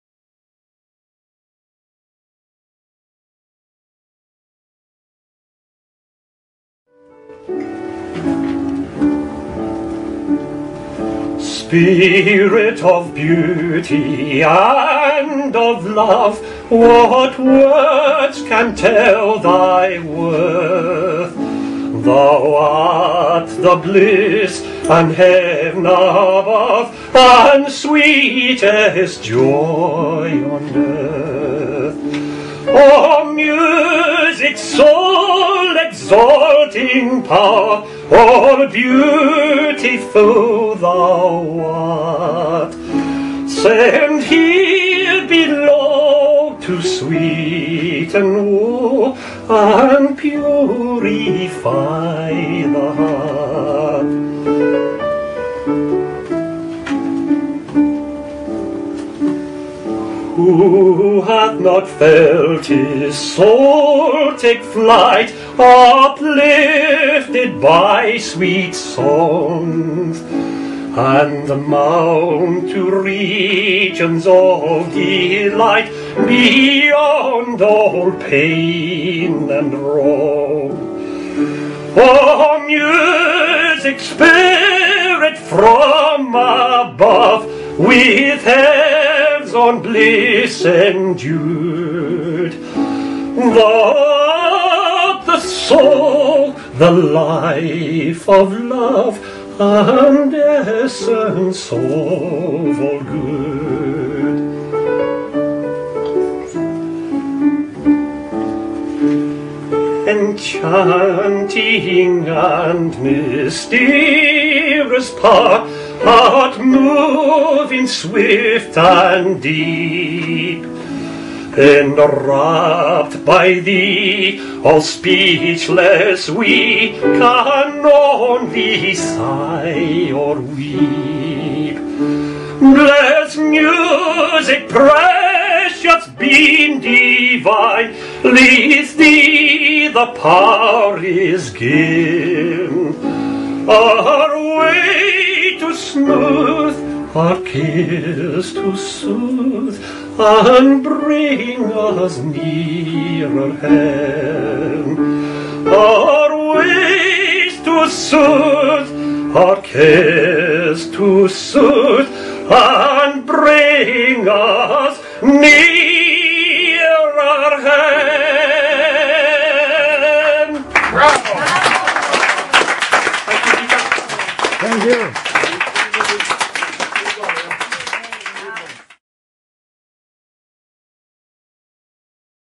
Australian Heritage Songs
Tom Black (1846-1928) wrote engaged poetry with deep meaning and was also a passionate defender of the rights of workers.
Here his verses are put to music to make them easier to understand for the modern fast paced public.